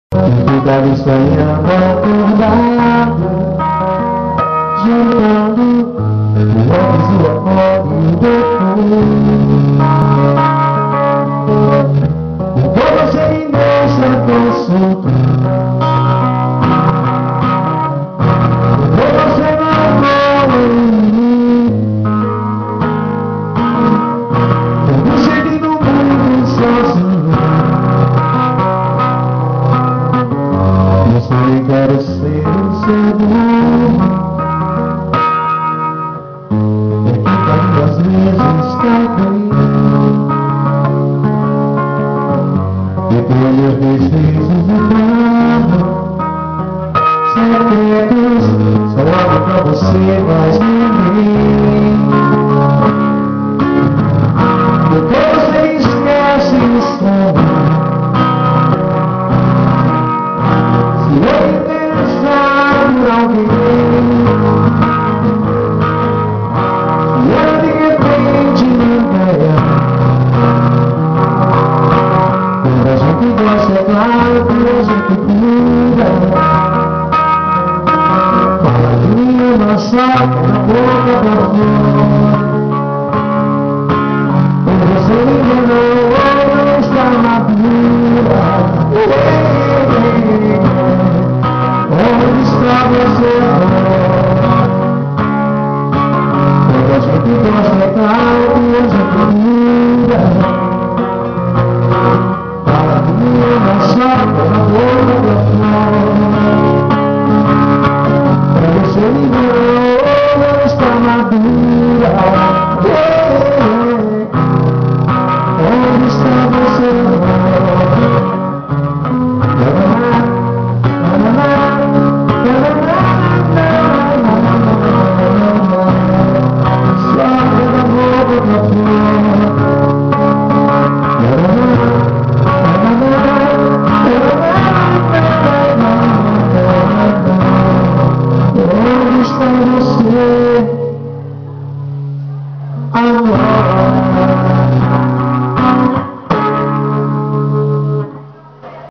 Jailson Siqueira Voz e Violão aovivo
Som aovivo com Jailson Siqueira na Fazenda Campus Ages de Jose Wilson em comemoração aos seus 50 anos.